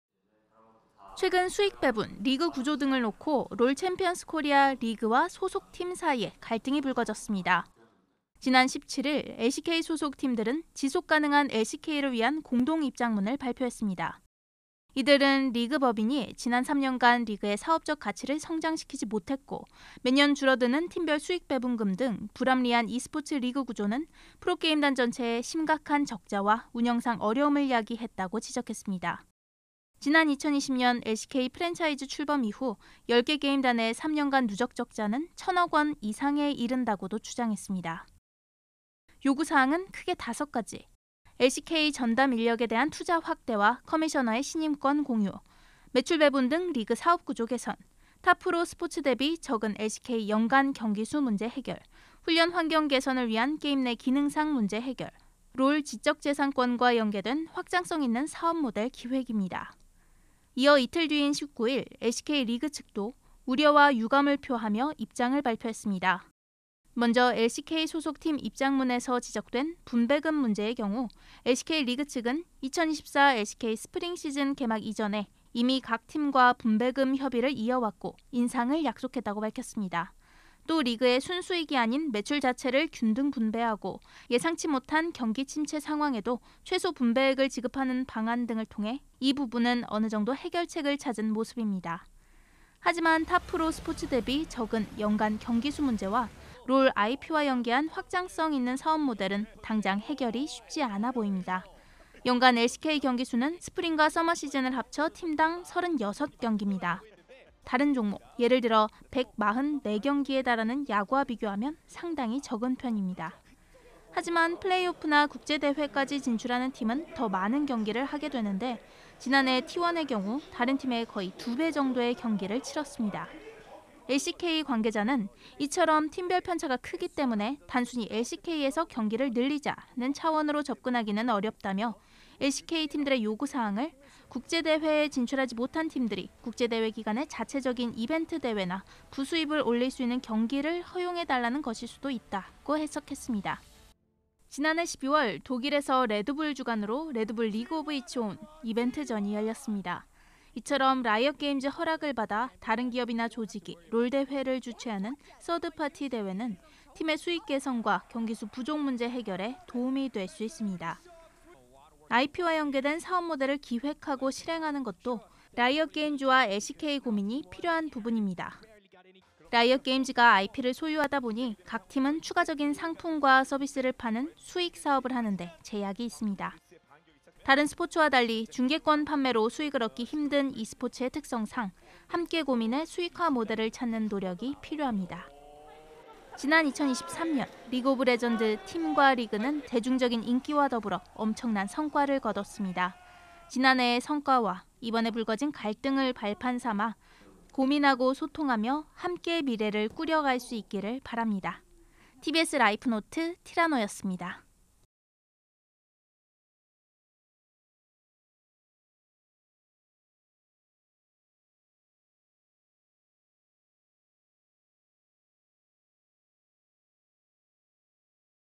공동입장문서 드러난 LCK 문제, 팀-리그 함께 고민해야｜티라노
최근 'LoL 챔피언스 코리아(LCK)' 리그와 소속 팀 사이에 갈등이 불거졌습니다. 수익 배분, 경기 수 문제, IP 수익화 모델 등 그간 누적되어온 문제가 터졌다는 반응이 이어졌는데요, 사실상 라이엇 게임즈가 결단을 해야 한다는 목소리가 나옵니다.

제작 티라노 (손승익 김희애 조주연 최인정)
자막 박은혜
영상 LCK 유튜브, Red Bull Gaming 유튜브